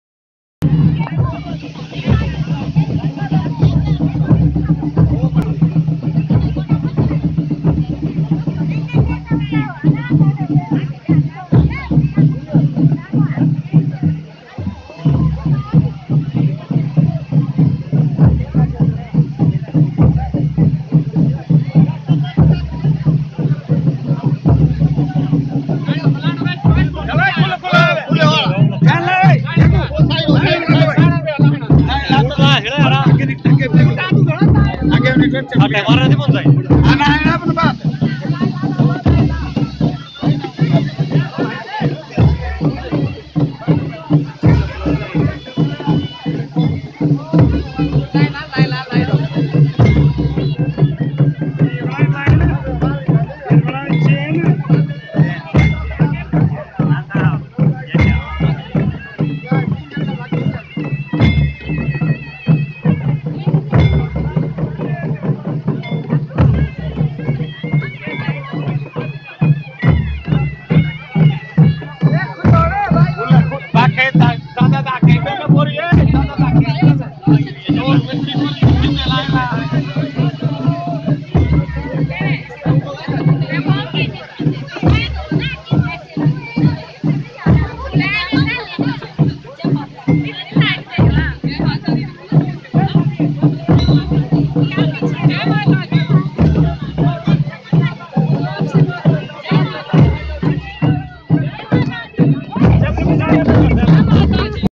Parvati mata sangam nahane at bhunter(Jia)
Mata chogasana(chong) sangam nahane...☺️ 09 july 2017